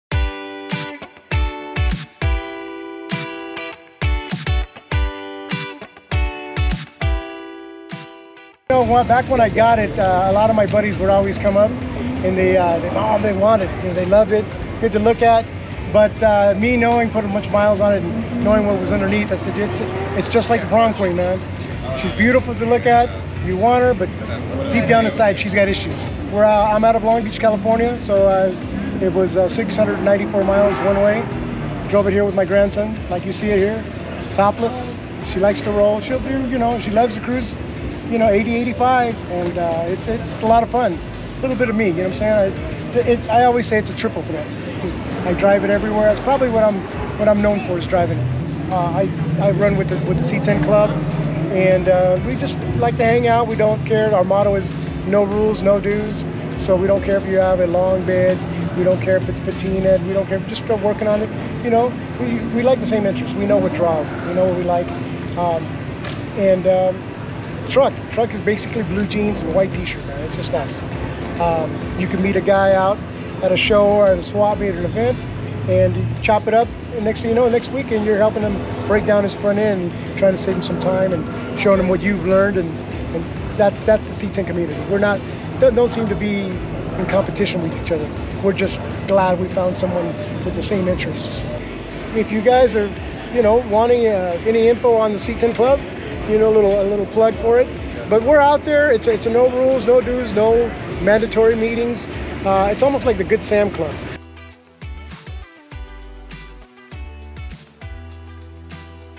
Classic of the Month: El Presidente Cruises With His '71 Blazer
Jon Oro, also known as El Presidente in the C10 community, has owned his '71 Blazer for 20 years. After several versions, Oro still loves driving it.